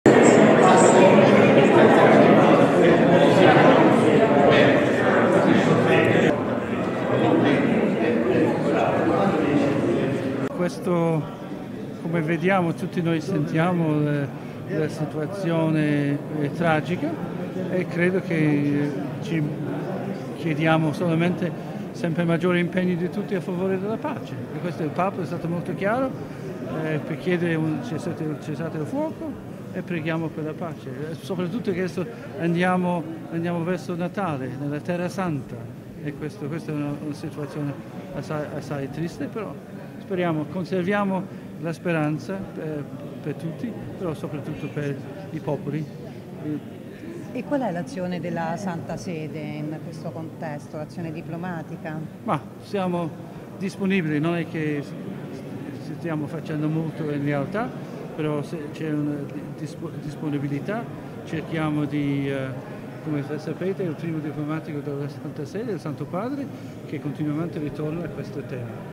MO, Gallagher: "Maggiore impegno per la pace. Papa chiaro su cessate il fuoco"
Il Segretario per i Rapporti con gli Stati della Santa Sede: "Triste Natale in Terra Santa"

POLITICA (Roma). "Tutti noi sentiamo questa tragica situazione e chiediamo solamente più impegno di tutto a favore della pace. Il Papa è stato chiaro per chiedere un cessate il fuoco e preghiamo per la pace. Soprattutto ora che andiamo verso Natale in Terra Santa. Conserviamo la speranza per tutti, soprattutto per i popoli. Siamo disponibili, cerchiamo di fare tutto: il primo diplomatico della Santa Sede è il Papa che continuamente ritorna su questo tema". Così Mons. Paul R. Gallagher, Segretario per i Rapporti con gli Stati della Santa Sede, a margine della Conferenza dei Missionari italiani dal titolo "La persona al centro" presso la Farnesina. (Marco Vesperini/alanews)

questo come vediamo tutti noi sentiamo adesso situazione è tragica e credo che chiediamo solamente sempre maggiore impegni di tutti è stato molto chiaro per chiedere fuoco e preghiamo per la pace e soprattutto che andiamo andiamo verso Natale nella terra santa e questo questa situazione Speriamo conserviamo la speranza per tutti soprattutto per i popoli della Santa Sede in questo contesto l'azione diplomatica siamo disponibili non è che  stiamo facendo molto alta però se c'è disponibilità cerchiamo di sapere il Santo Padre che ritorno è questo te